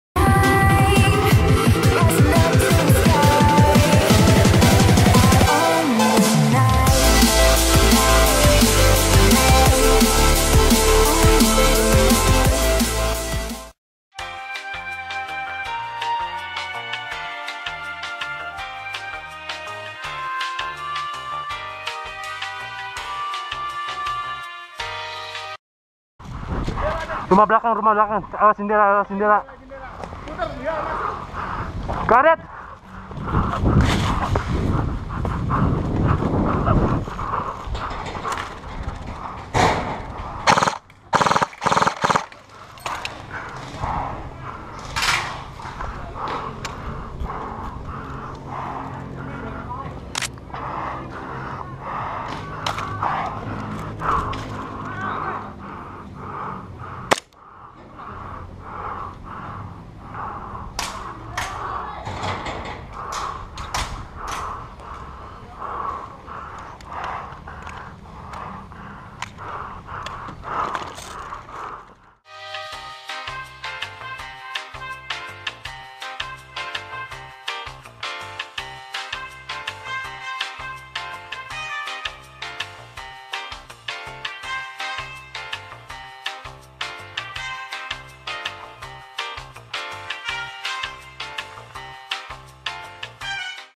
new switching animations for later airsoft videos
hey guys. just want to show the new animation that i'm gonna use for the next videos.
i use my old video for this.